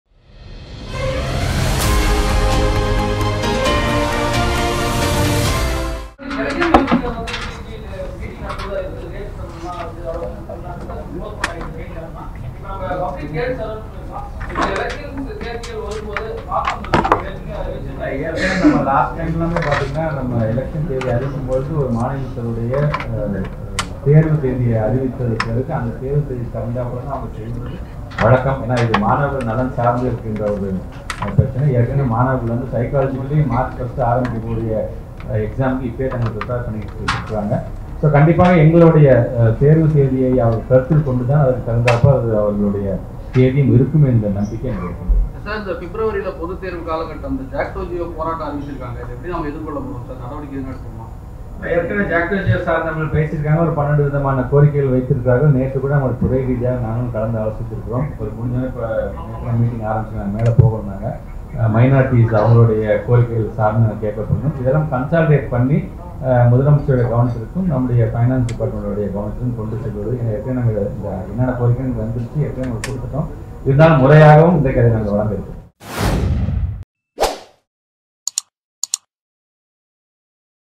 “பொதுத்தேர்வு அட்டவணையில் மாற்றம்?”:அமைச்சர் அன்பில் மகேஷ் விளக்கம் | Dinamani
Dinamani Daily Seithigal Morning:
Dinamani Daily Seithigal Evening:
Dinamani Daily Breaking News:
Hourly Headlines: